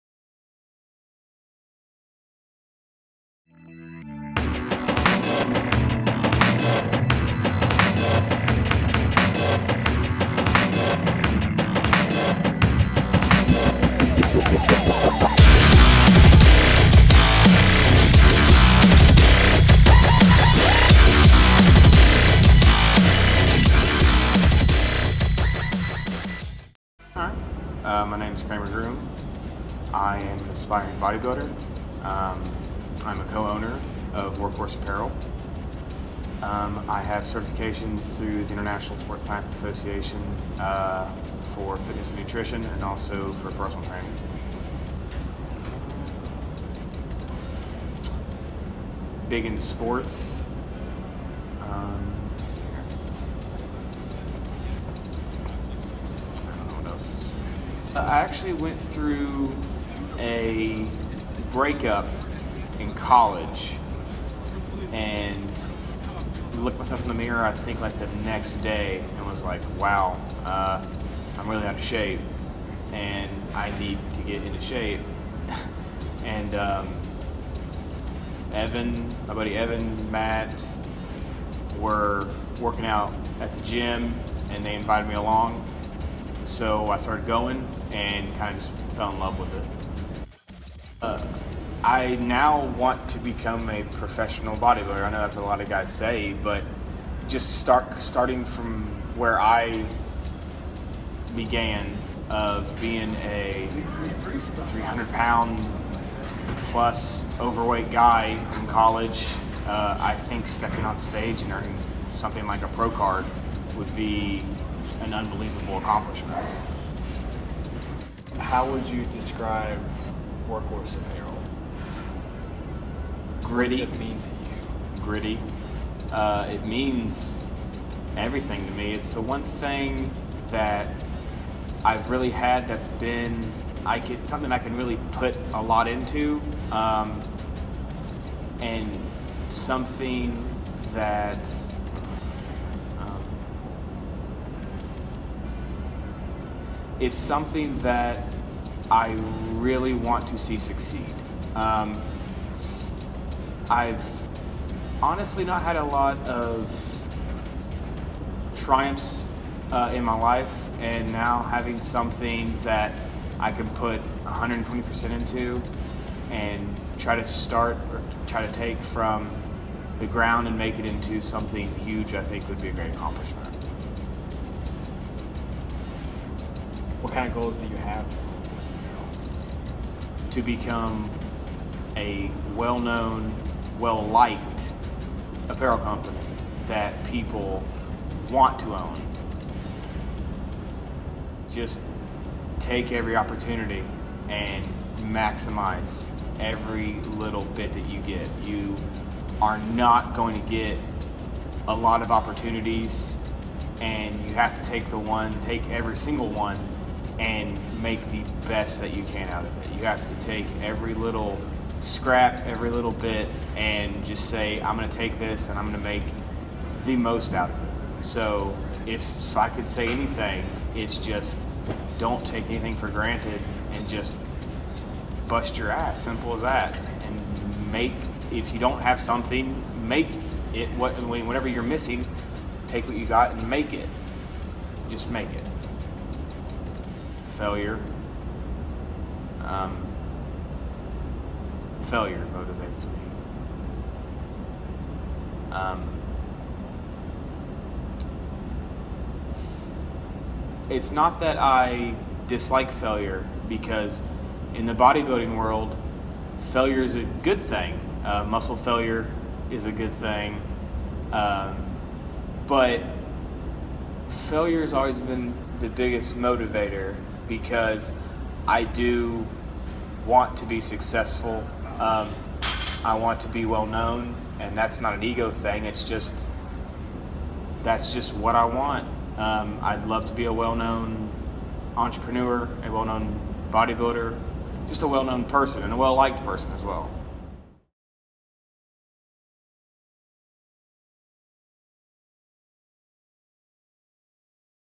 Meet Kramer Groom
Meet one of the men behind WorkHorse Apparel. But, more importantly, meet a man who has overcome many obstacles.

             Korn (feat. Skrillex & Kill the Noise)